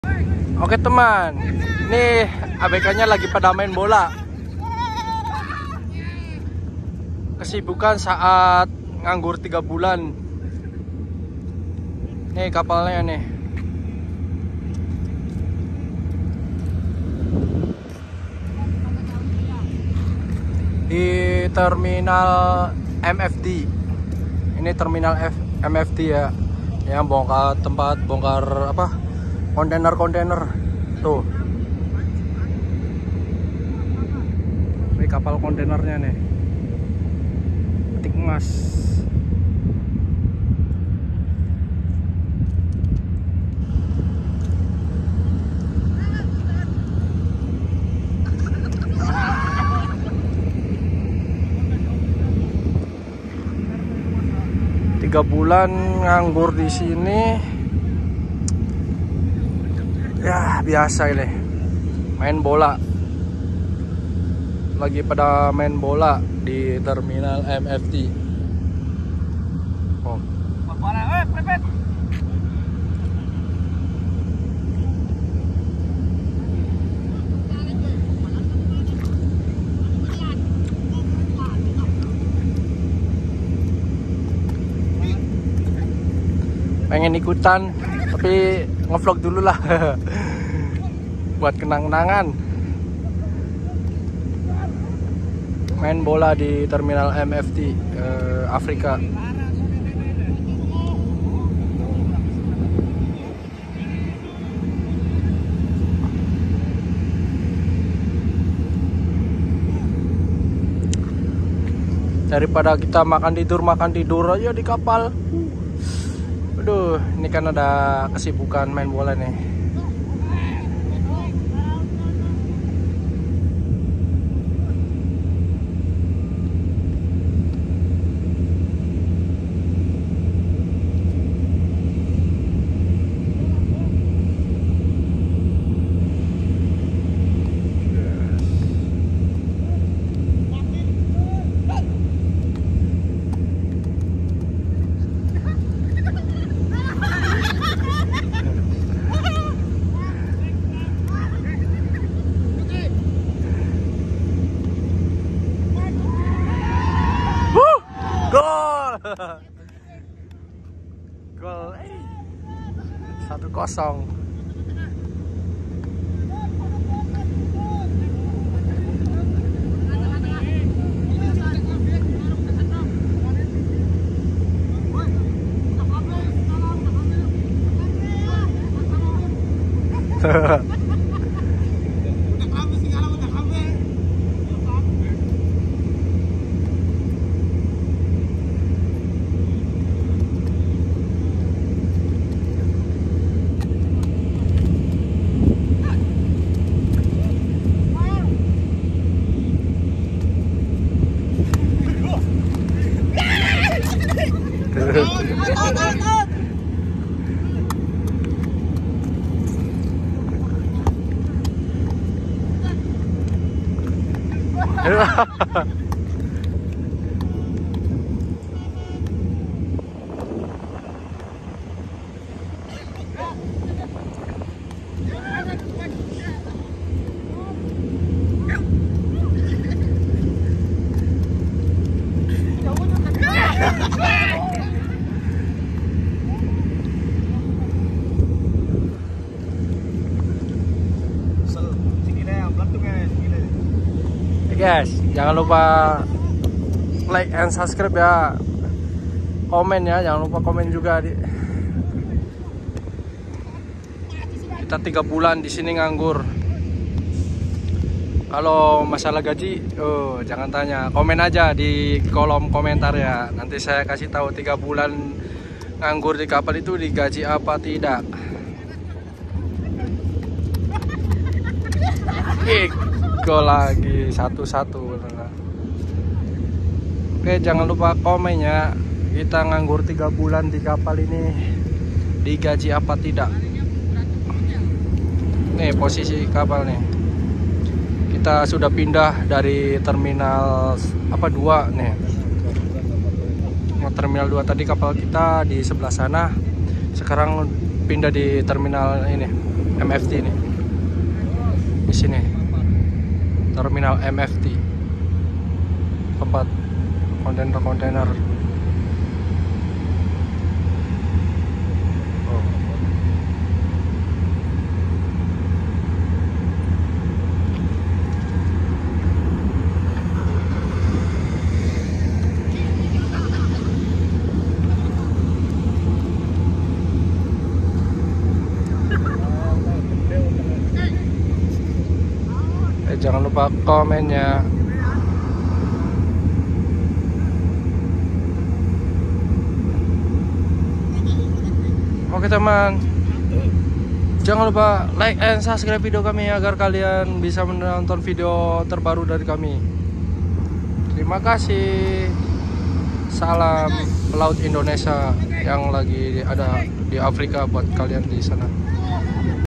MAIN BOLA DI PELABUHAN MFD AFRICA
Hey teman kita disini di africa, kita lagi main bola nih dari pada kita jenuh makan tidur terus 3bulan nganggur lebih baik main bola biar ada tenagaan.. yuk jangan lupa lik and subscribe ya viedo kami agar kalian bisa menonton video terbaru dari kami. Salam jangkar pelaut indonesia cheppy coffee.